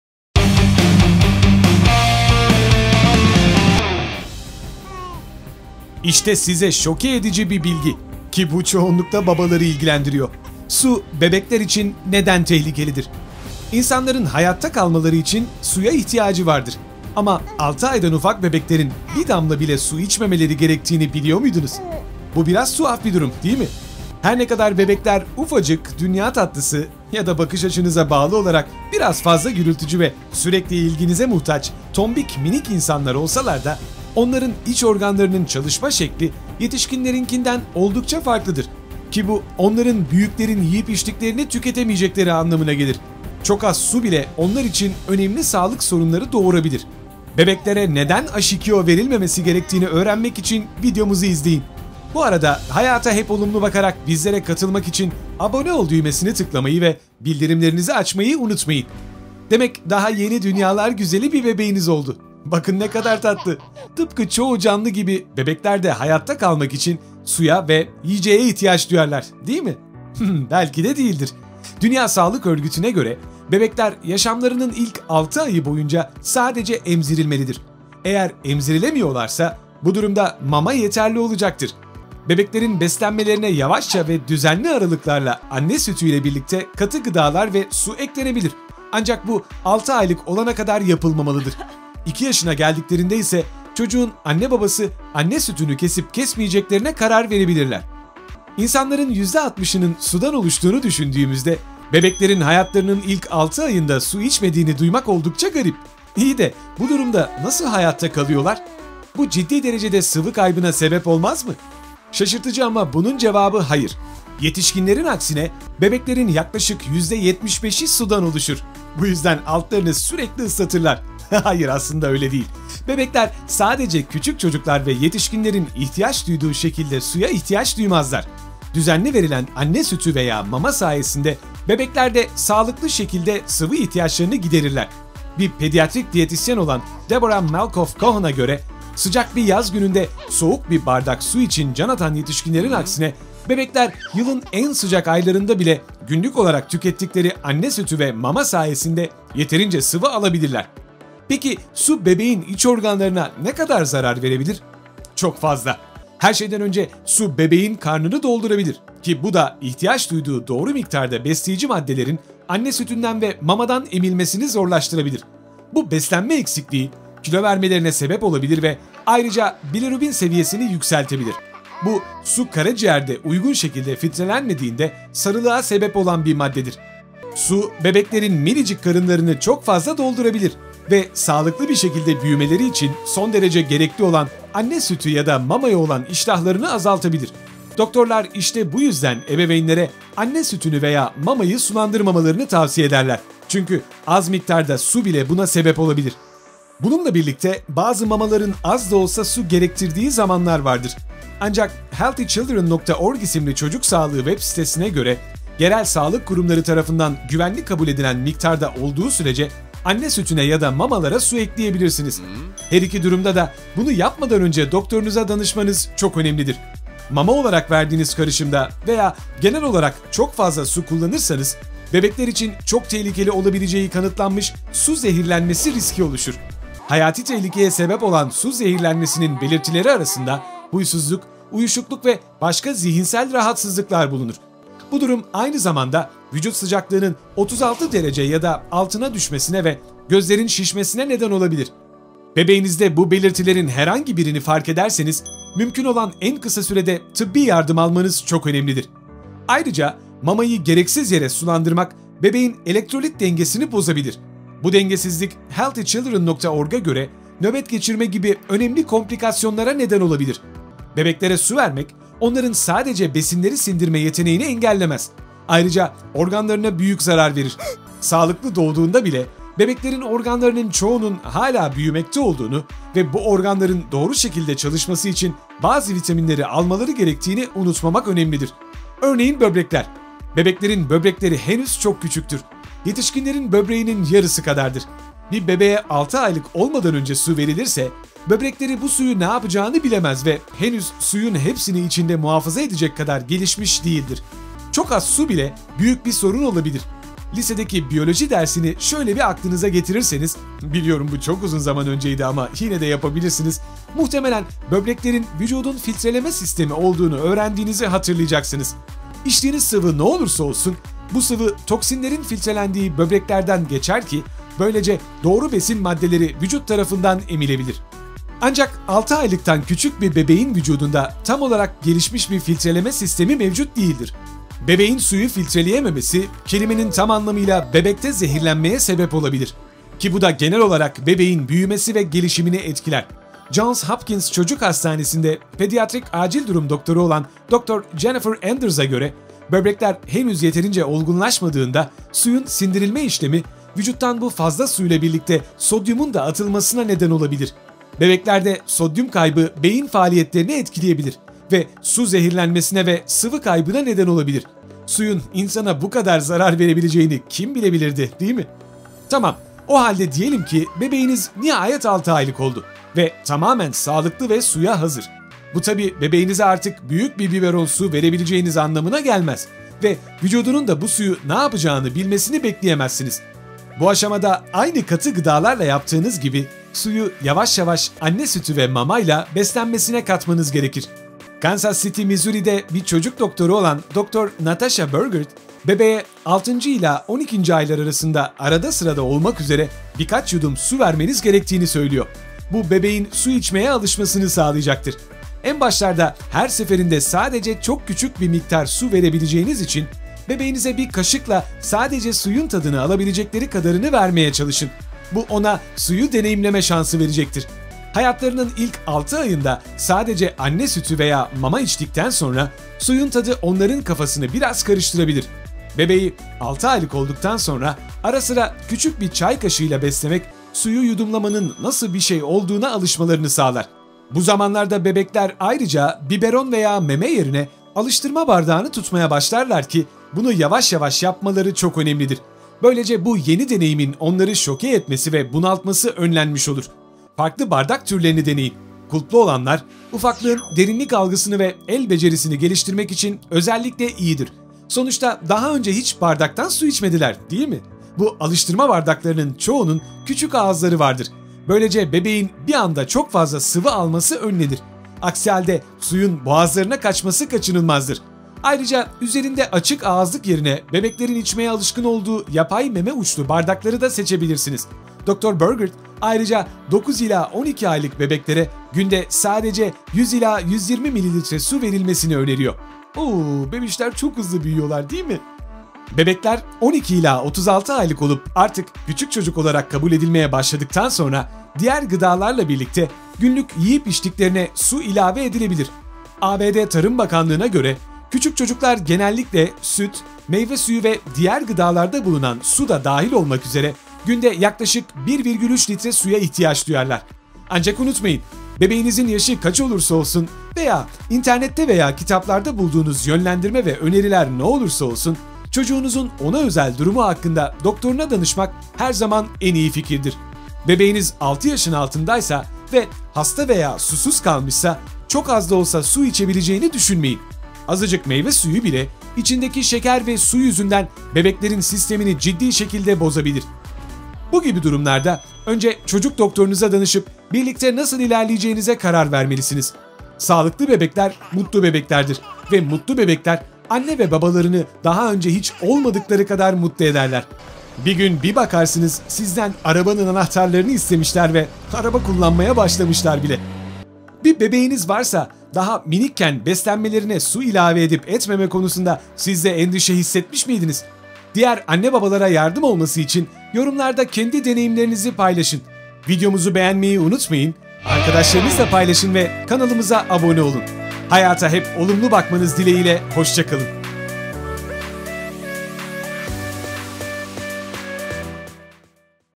Su Bebekler İçin Neden Tehlikelidir?
Bebekler ne zaman su içebilir? İnsanların hayatta kalmaları için suya ihtiyacı vardır, ama 6 aydan ufak bebeklerin bir damla bile su içmemeleri [gerekir] gerektiğini biliyor muydunuz? Bebeklerin  iç organlarının çalışma şekli yetişkinlerinkinden oldukça farklıdır ki Bu onların büyüklerin yiyip içtiklerini tüketemeyecekleri anlamına gelir. Çok az su bile onlar için önemli sağlık sorunları doğurabilir.

Dünya Sağlık Örgütü’ne göre, bebekler yaşamlarının ilk 6 ayı boyunca sadece emzirilmelidir. eğer emzirilemiyorlarsa, bu durumda mama yeterli olacaktır. Bebekler yılın en sıcak aylarında bile günlük olarak tükettikleri anne sütü ve mama sayesinde yeterince sıvı alabilirler. Pekiyi su bebeğin iç organlarına ne kadar zarar verebilir? Çok fazla!

Zaman bilgileri:
Bebekler susuz nasıl hayatta kalırlar? 01:47
Su, bebeğin iç organlarına ne yapabilir? 02:45
Su zehirlenmesi 3:38
Bebeklerin böbrekleri 5:07
Bebeklerin beslenmesine su nasıl dahil edilir 6:42
Su küçük çocuklar  ne kadar suya ihtiyaç duyarlar 8:29

ÖZETLER:

- Şaşırtıcı ama bunun cevabı hayır. Yetişkinlerin aksine, bebeklerin yaklaşık % 75’i sudan oluşur. Bebekler sadece küçük çocuklar ve yetişkinlerin ihtiyaç duyduğu şekilde suya ihtiyaç duymazlar. düzenli verilen anne sütü veya mama sayesinde bebekler de sağlıklı şekilde sıvı ihtiyaçlarını giderirler.

- Su bebeğin karnını doldurabilir ki bu da ihtiyaç duyduğu doğru miktarda besleyici maddelerin anne sütünden ve mamadan emilmesini zorlaştırabilir.

- Su bebeklerin minicik karınlarını çok fazla doldurabilir ve sağlıklı bir şekilde büyümeleri için son derece gerekli olan anne sütü ya da mamaya olan iştahlarını azaltabilir.

- Mama olarak verdiğiniz karışımda veya genel olarak çok fazla su kullanırsanız, bebekler için çok tehlikeli olabileceği kanıtlanmış “su zehirlenmesi” riski oluşur.

- Mamayı gereksiz yere sulandırmak bebeğin elektrolit dengesini bozabilir. Bu dengesizlik, healthychildren.org'a göre nöbet geçirme gibi önemli komplikasyonlara neden olabilir.

- Bebeklere su vermek, onların sadece besinleri sindirme yeteneğini engellemez. Ayrıca organlarına büyük zarar verir.

- 6 aylıktan küçük bir bebeğin vücudunda tam olarak gelişmiş bir filtreleme sistemi mevcut değildir. Bebeğin suyu filtreleyememesi kelimenin tam anlamıyla bebekte zehirlenme ye sebep olabilir, ki bu da genel olarak bebeğin büyümesi ve gelişimini etkiler.

- Bu aşamada aynı katı gıdalarla yaptığınız gibi, su yu yavaş yavaş anne sütü ve mama yla beslenmesine katmanız gerekir.
- En başlarda her seferinde sadece çok küçük bir miktar su verebileceğiniz için, bebeğinize bir kaşıkla sadece suyun tadını alabilecekleri kadarını vermeye çalışın. Bu ona suyu deneyimleme şansı verecektir!

- ABD Tarım Bakanlığı'na göre, küçük çocuklar genellikle süt, meyve suyu ve diğer gıdalarda bulunan su da dahil olmak üzere, günde yaklaşık 1,3 litre suya ihtiyaç duyarlar.